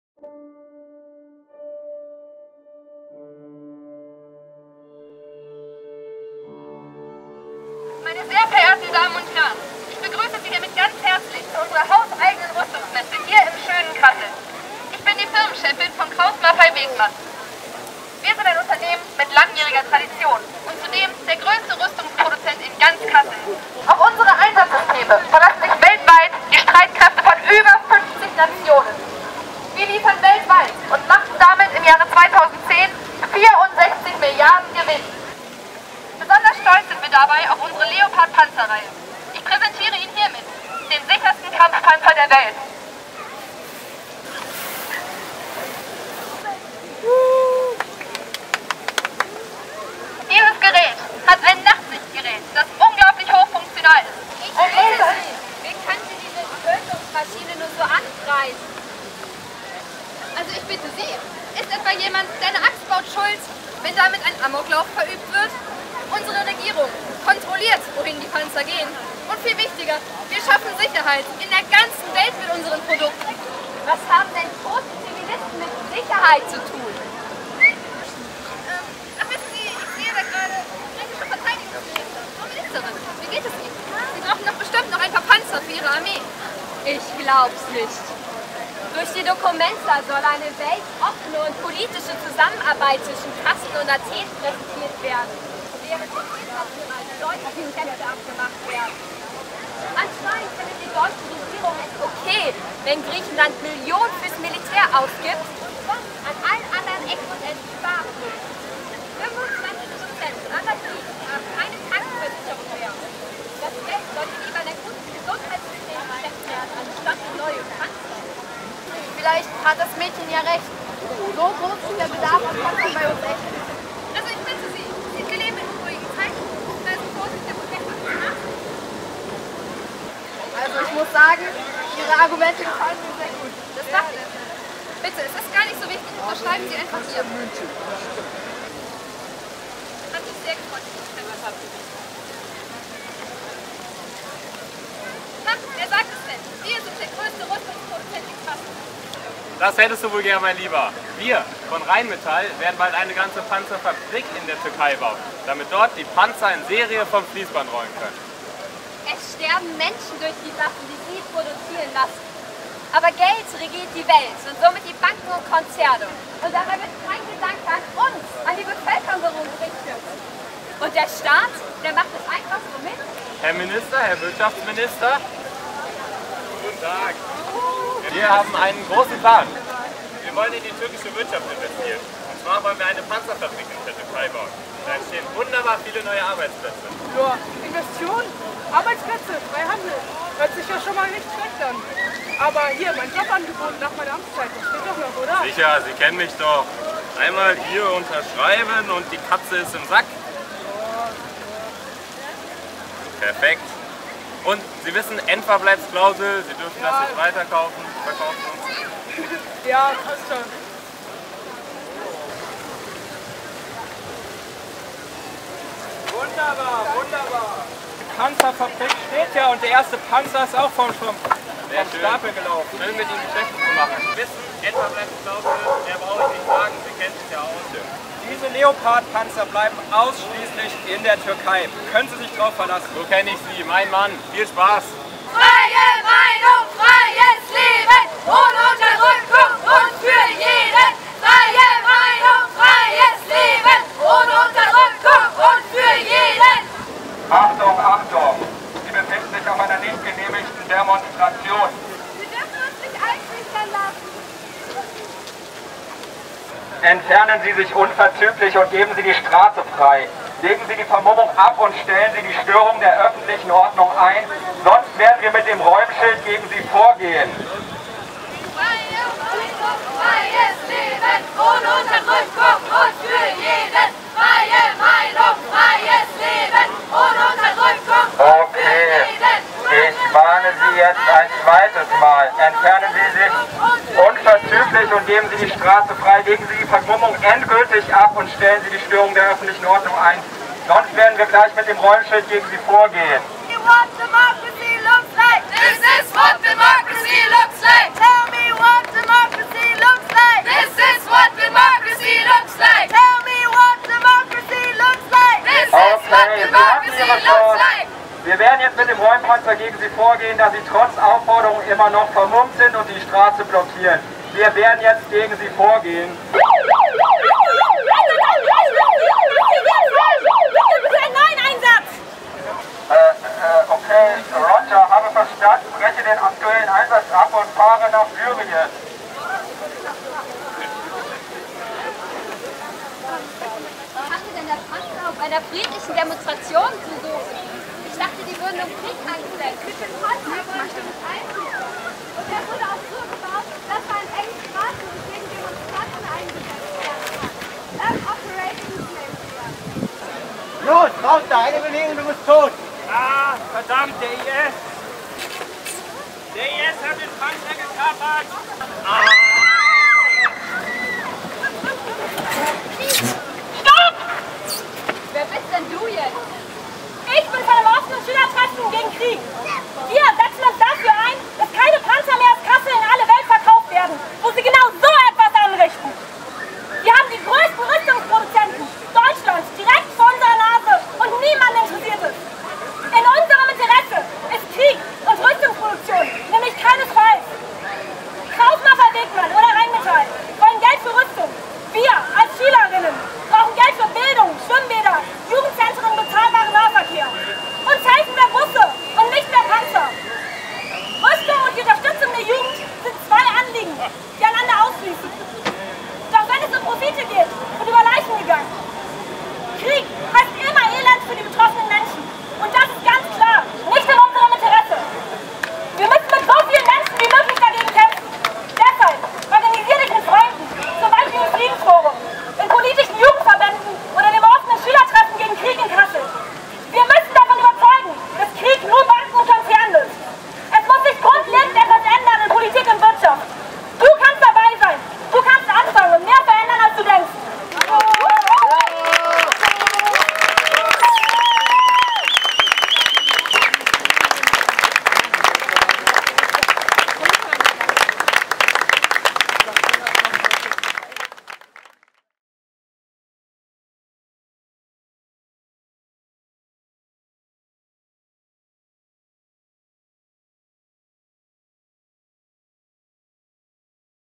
Waffen wandern - Straßentheater gegen Rüstungsexporte in Kassel
Panzer mitten in Kassel produziert!?
Leider immernoch bittere Realität.
Wie die Panzer den Weg um die Welt finden, wo und gegen wen sie eingesetzt werden, hat das Offene Schülertreffen gegen Krieg in einem kleinen Straßentheater deutlich gemacht.